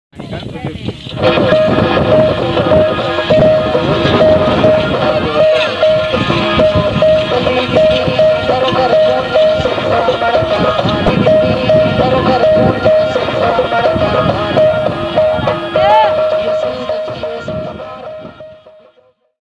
Camal cart in Thar for water fetching |Thar Beauty
Thari people uses the camel cart for water fetching, the people of thar facing sever scarcity of water.